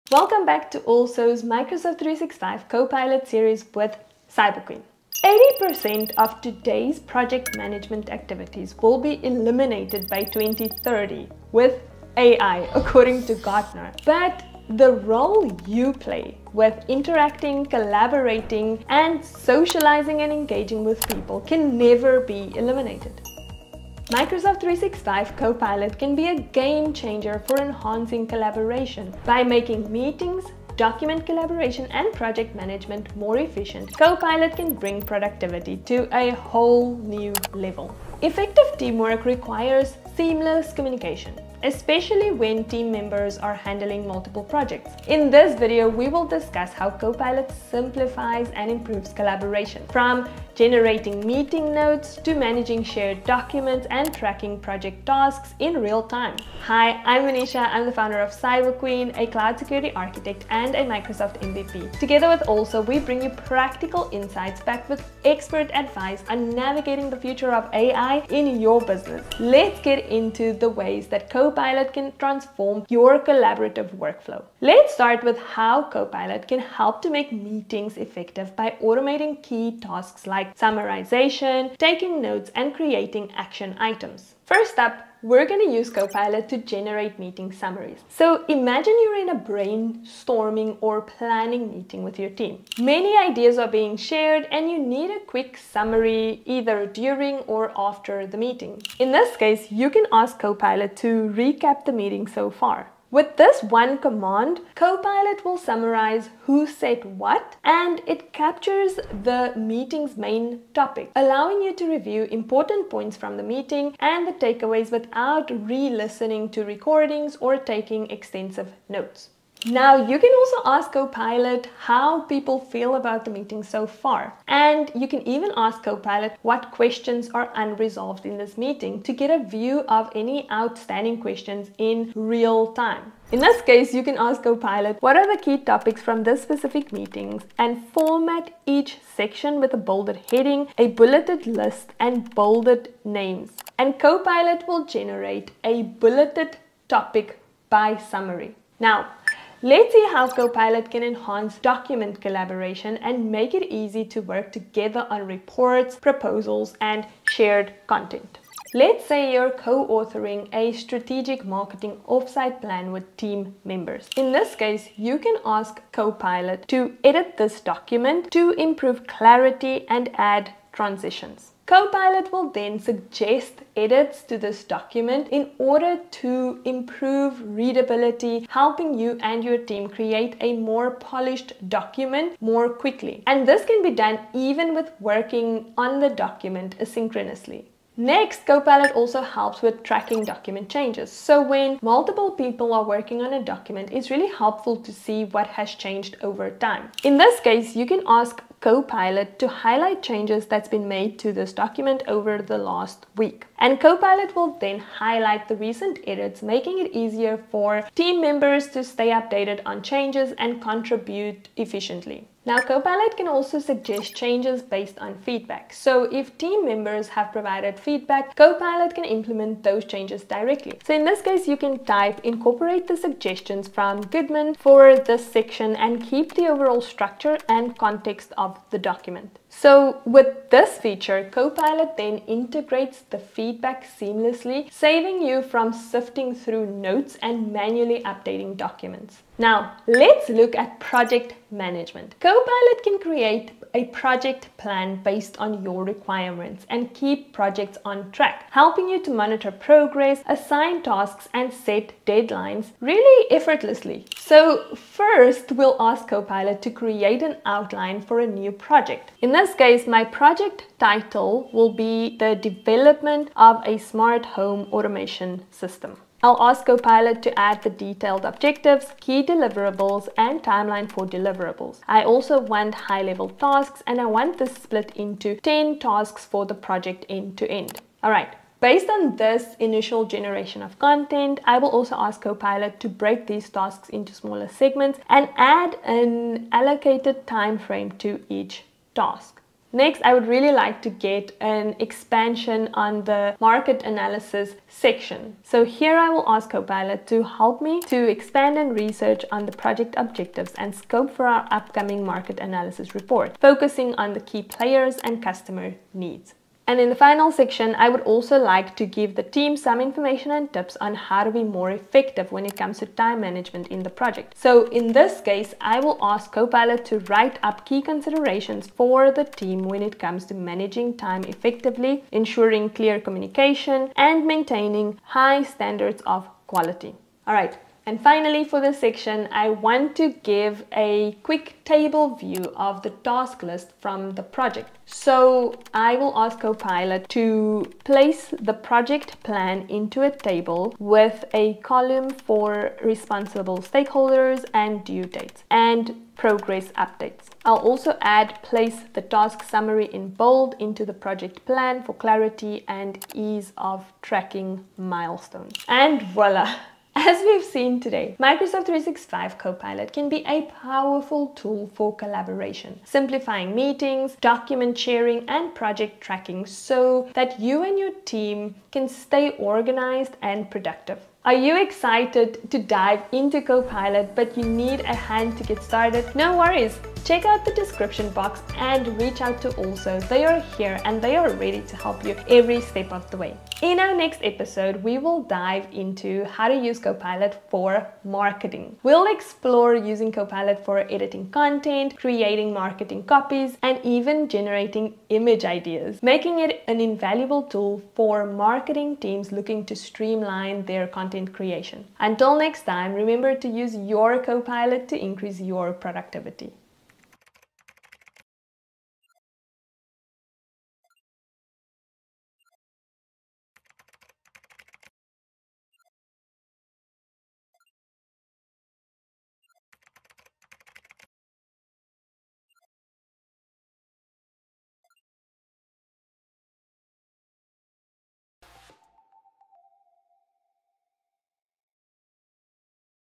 Video 8 - Enhancing Collaboration with Copilot
Discover the exciting AI opportunities and Copilot’s transformative technology for Microsoft 365 and join us for webinars on Microsoft 365 Copilot

Next videos:
How to use Copilot for Marketing
Copilot for Data Analysis and Reporting